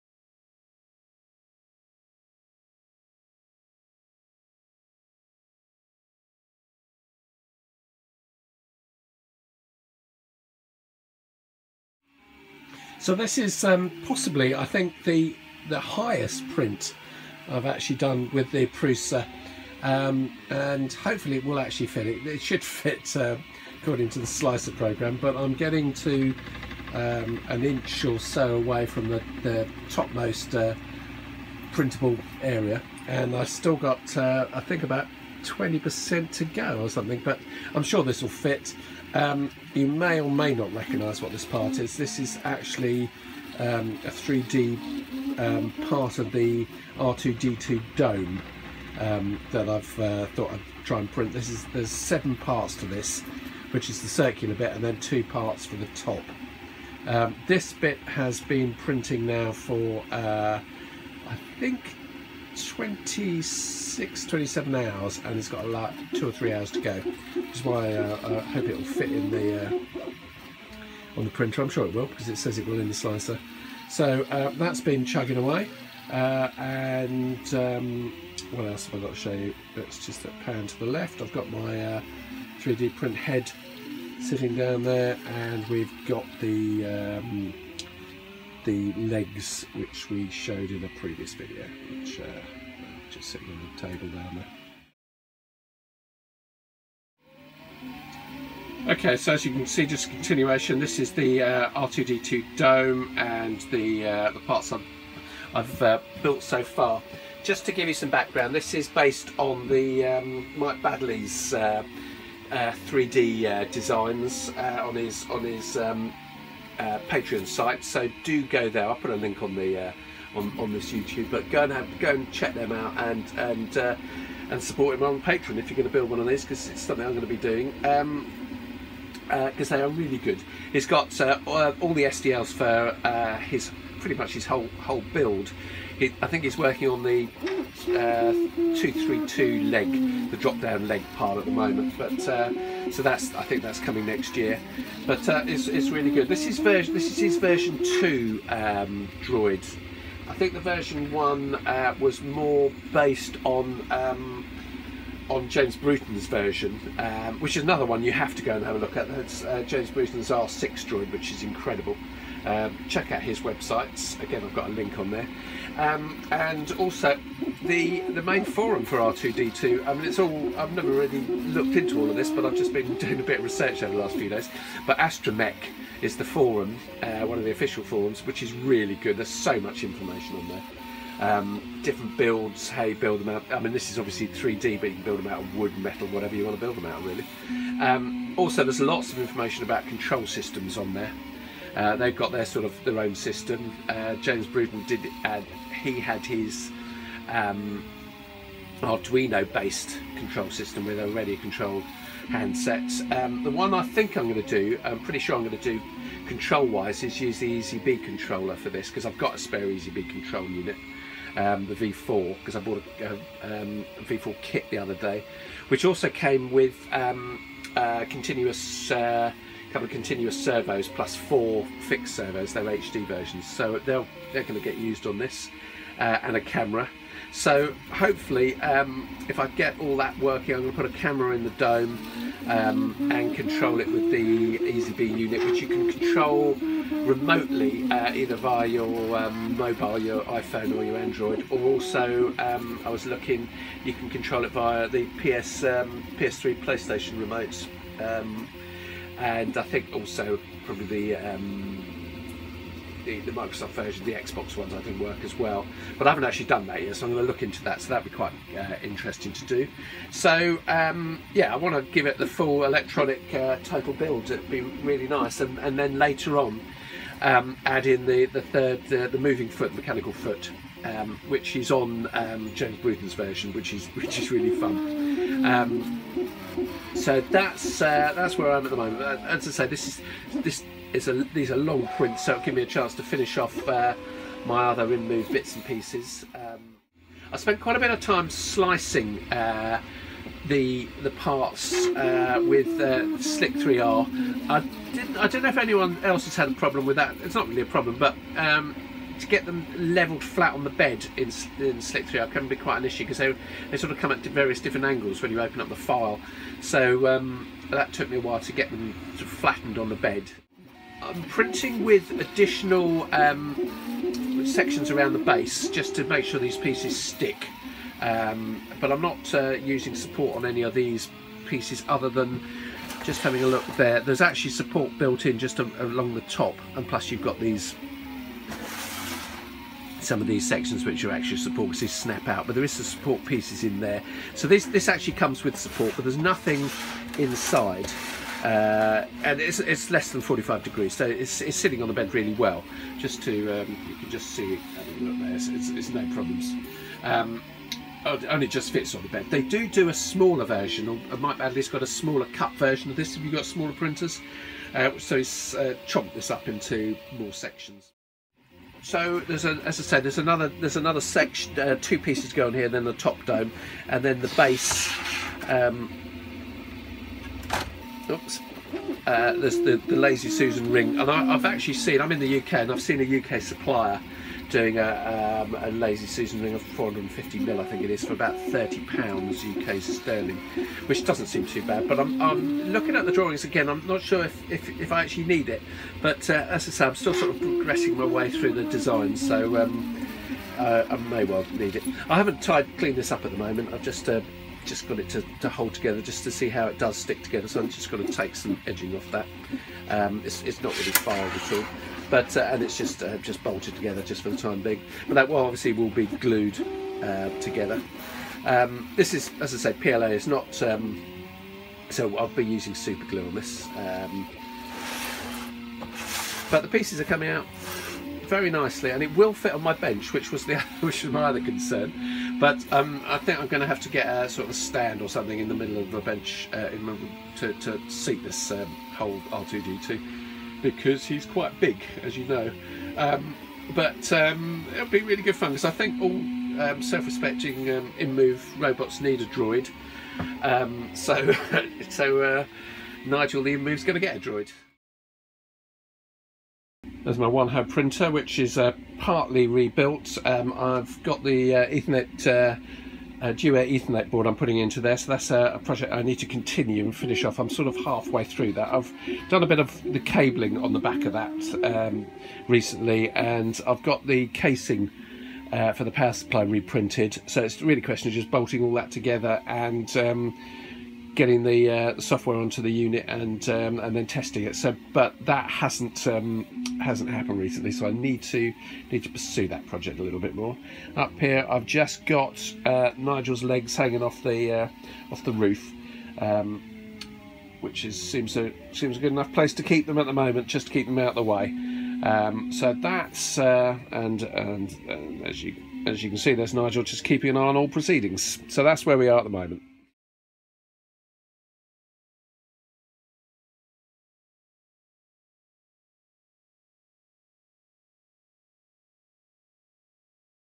R2D2 Droid Build Part - 1
An R2D2 droid as a companion for Nigel my InMoov robot.
This is part one of a series of builds

This is a 3D print using Michael Baddeleys excellent STL files